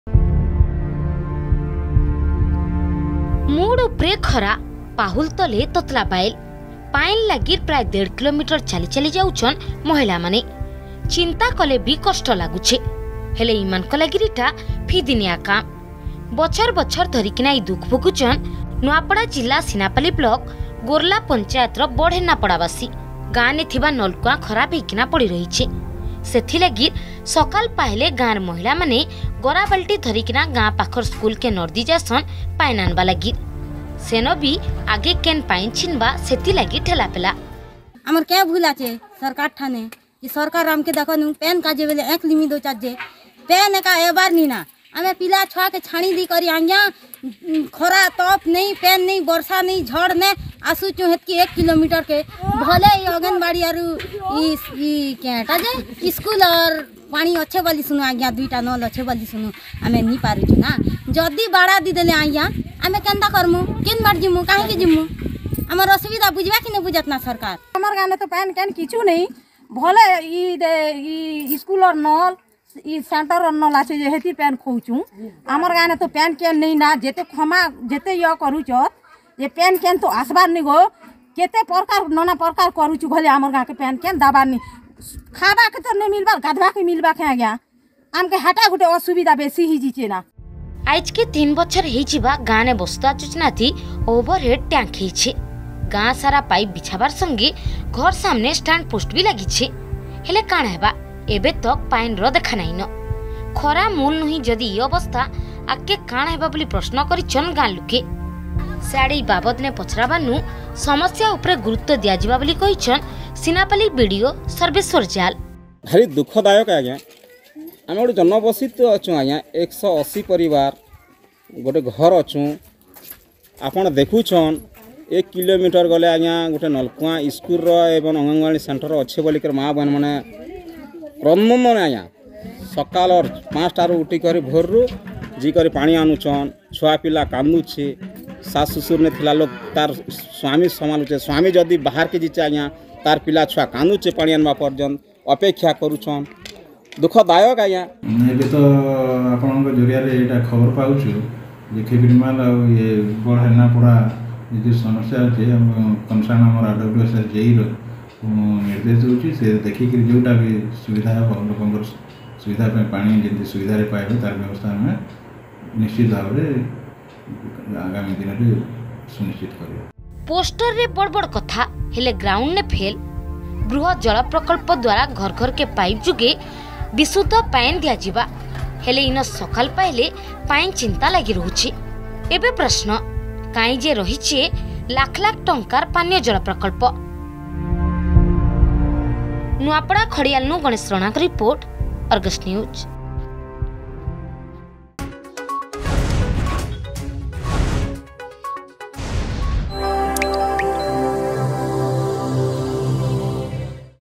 ପୋଷ୍ଟର୍‌ ବଡ୍, ଗ୍ରାଉଣ୍ଡନେ ଫେଲ୍
ପୋଷ୍ଟରରେ ବଡ ବଡ କଥା ହେଲେ ଗ୍ରାଉଣ୍ଡନେ ଫେଲ୍ । ବୃହତ ଜଳ ପ୍ରକଳ୍ପ ଦ୍ବାରା ଘର ଘର୍‌କେ ପାଇପ ଯୁଗେ ବିଶୁଦ୍ଧ ପାଏନ୍ ଦିଆଯିବା। ହେଲେ ନୂଆପଡା ଜିଲ୍ଲା ସିନାପାଲି ବ୍ଲକନେ ସକାଲ୍ ପାହେଲେ ପାଏନ୍ ଚିନ୍ତା । ଇନ ପ୍ରଶ୍ନ କାଏଁଯେ ଆର୍ କାହାର୍ ଲାଗିର୍ ଲାଖ୍ ଲାଖ୍ ଟଁକାର୍ ପାନୀୟ ଜଳ ପ୍ରକଳ୍ପ ?